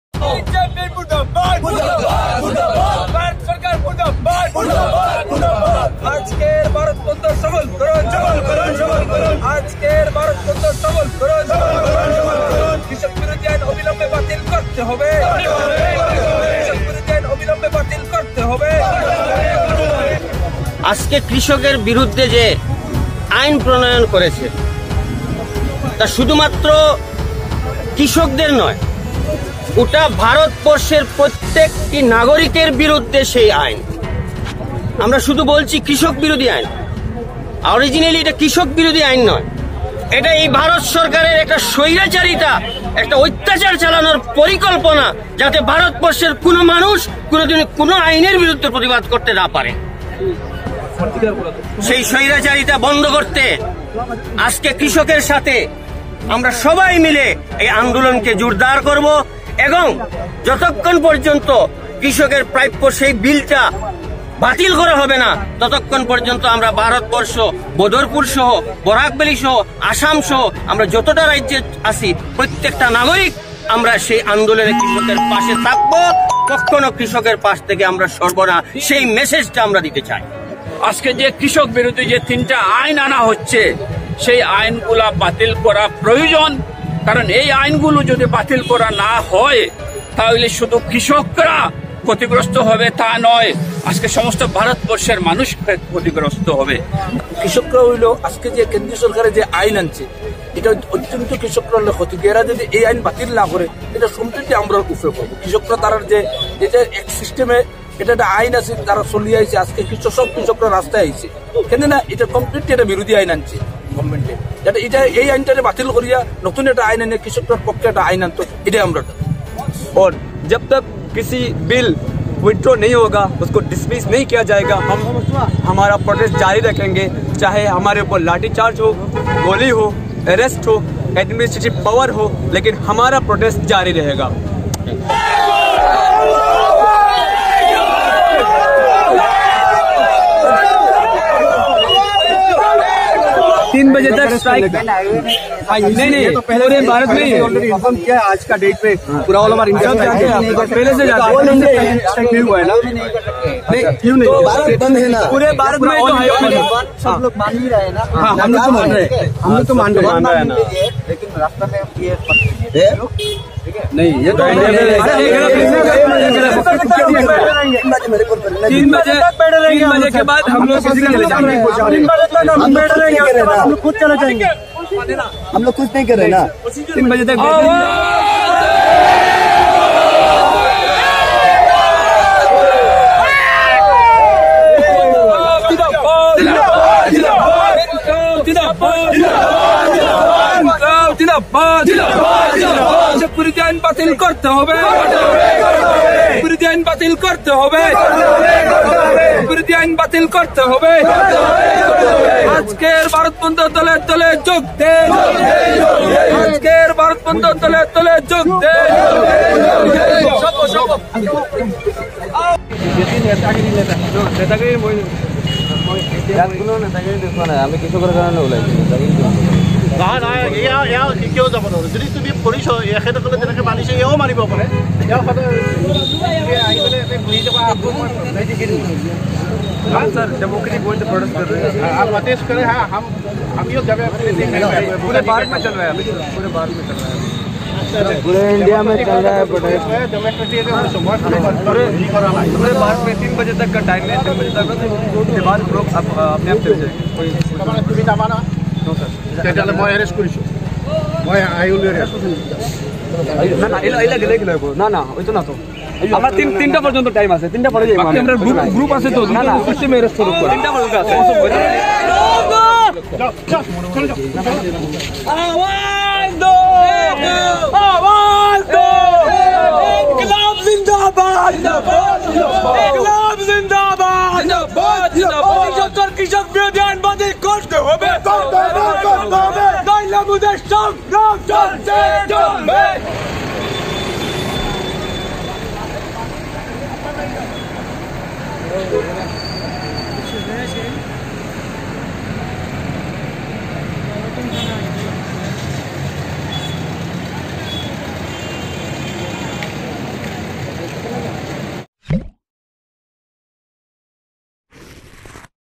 ভারত বনধ : কৃষি আইনের বিরুদ্ধে উত্তাল বদরপুর
ভারত বনধ : কৃষি আইনের বিরুদ্ধে উত্তাল বদরপুর! জাতীয় সড়ক অবরোধ করে প্রতিবাদ